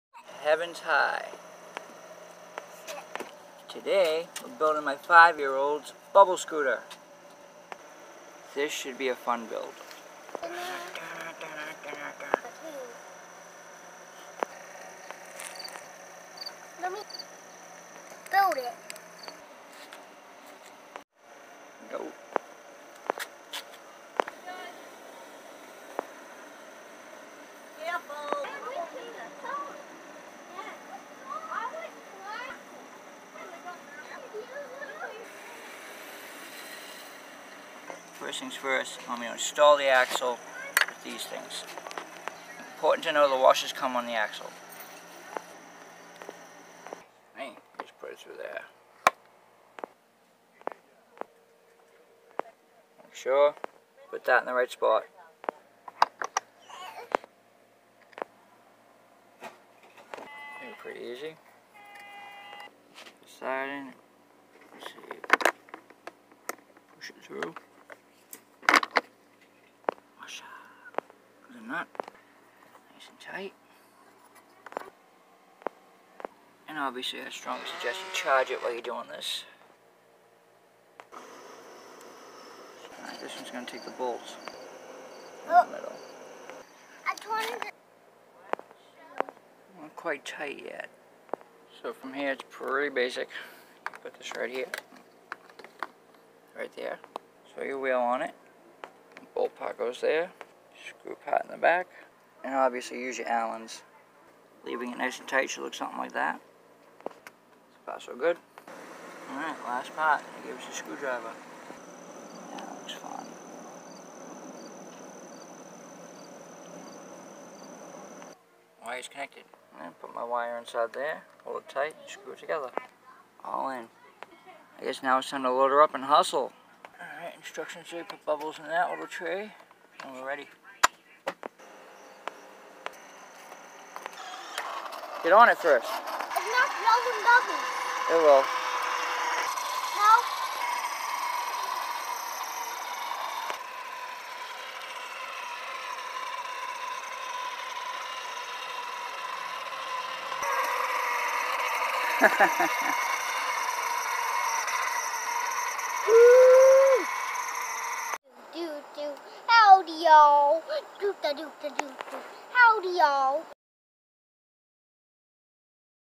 Bubble scooter build, EASY.
Building a kids electric bubble blowing scooter using the simple easy steps in the instruction booklet provided. HUFFY 2-in-1 V6-Battery or kick power scooter that makes bubbles as your kid rides it. (Ages 3-5 Max Wt·50 lbs.) 2.0 MPH w/A 40min battery.
Name brand puppies to the rescue.